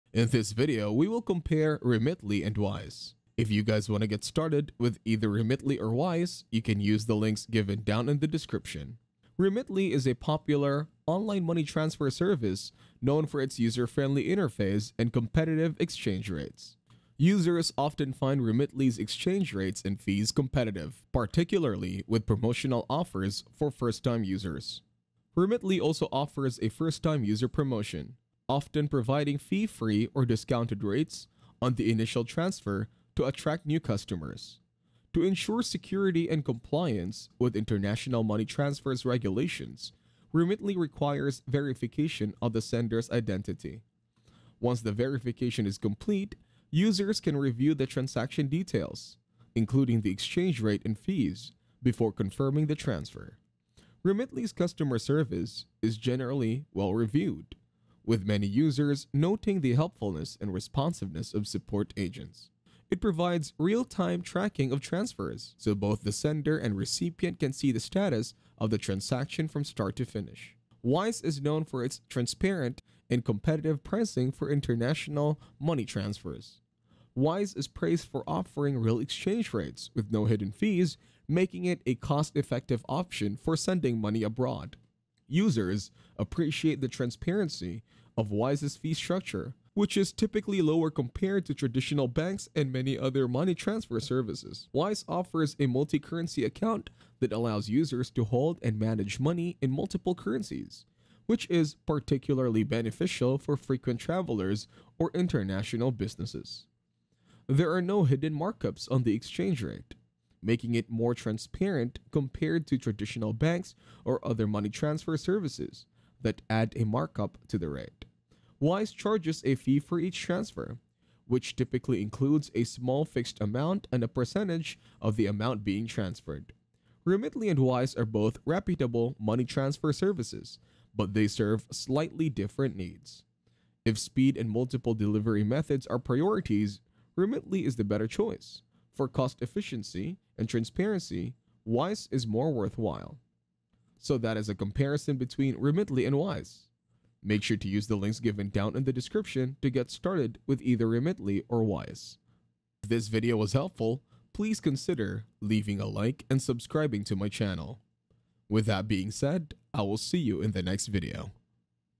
Remitly vs Wise (2025) | Which is Better?
In this video I'll compare Remitly vs Wise